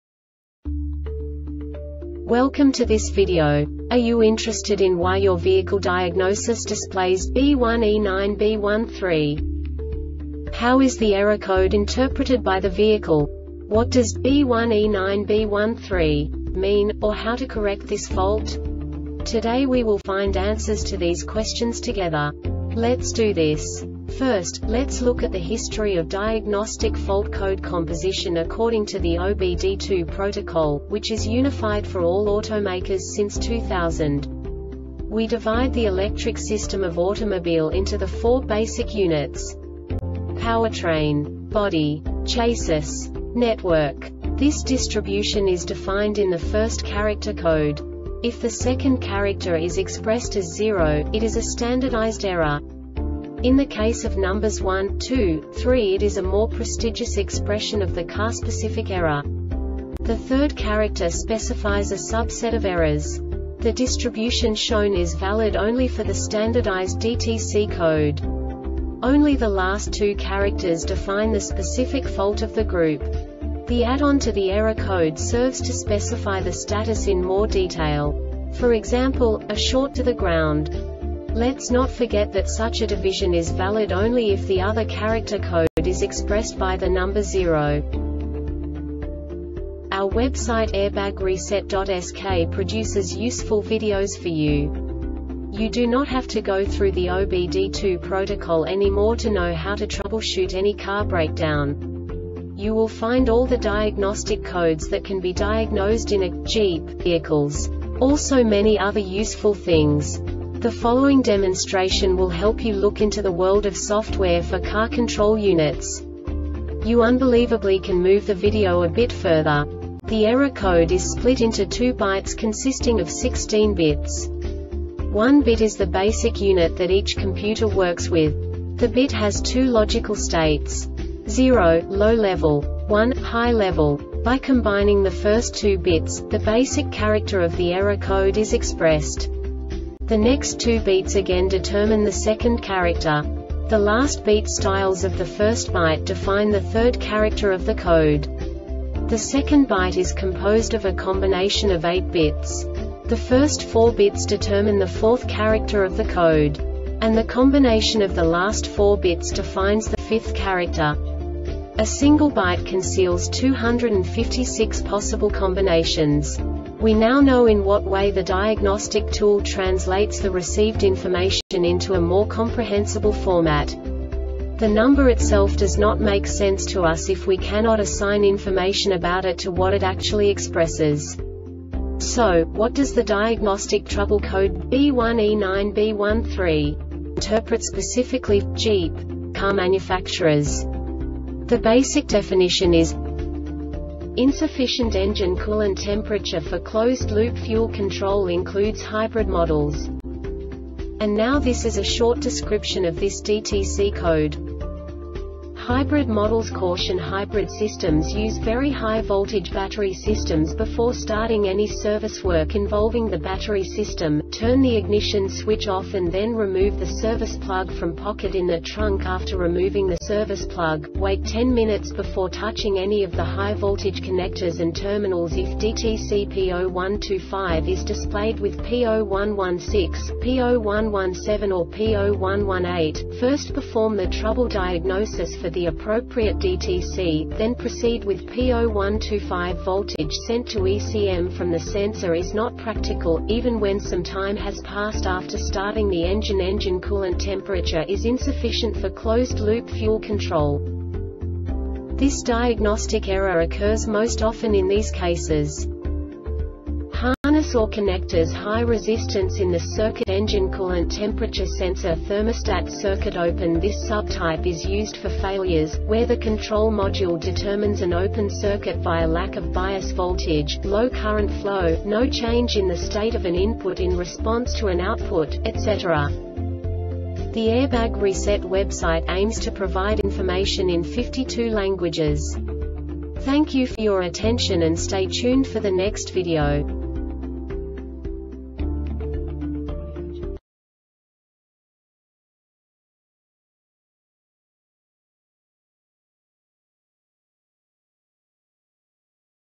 DTC Jeep B1E9B-13 Short Explanation
Contents:
0:21 Basic DTC analysis according to OBD2 protocol standard.
1:48 Insight into programming
2:58 A diagnostically understandable description of the Diagnostic Trouble Code
3:05 Basic error definition

Make:
Jeep
Code:
B1E9B 13
Definition:
Insufficient Engine Coolant Temperature for Closed Loop Fuel Control (Includes Hybrid Models)
Description:
HYBRID MODELS CAUTION: Hybrid systems use very high-voltage battery systems.Before starting any service work involving the battery system,turn the ignition switch OFF and then remove the service plug from pocket in the trunk.After removing the service plug,wait 10 minutes before touching any of the high-voltage connectors and terminals. If DTC P0125 is displayed with P0116, P0117 or P0118, first perform the trouble diagnosis for the appropriate DTC,then proceed with P0125.Voltage sent to ECM from the sensor is not practical,even when some time has passed after starting the engine. Engine coolant temperature is insufficient for closed loop fuel control.
Cause:
 Harness or connectors- High resistance in the circuit Engine coolant temperature sensor Thermostat
Failure Type:
Circuit Open